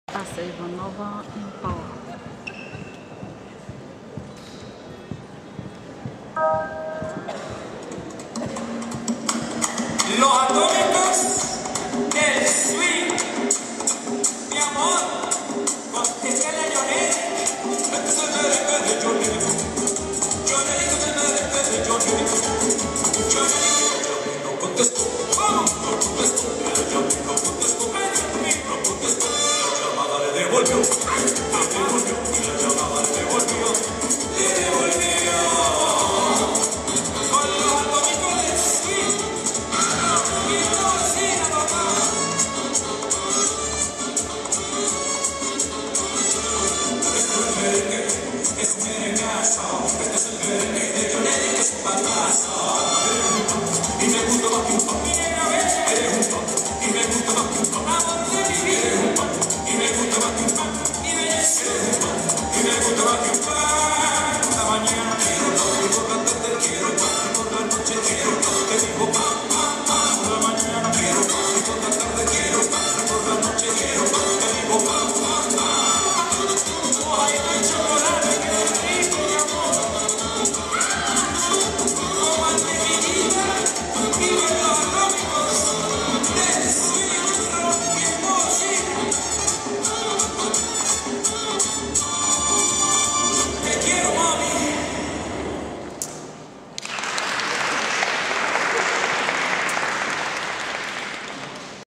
ASYA IVANOVA (Hoop)-2017 NT "J. Shishmanova" (Seniors-Qualifications-Hoop+Clubbs)
Държавно Първенство "Жулиета Шишманова" (Ден втори)-Квалификации (Обръч и Бухалки) при Жените. Бургас, Сп. Зала "МЛАДОСТ" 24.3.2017г.
RG National Tournament "Julieta Shishmanova" (Seniors-Qualifications-Hoop and Clubbs), Bourgas 24 March 2017.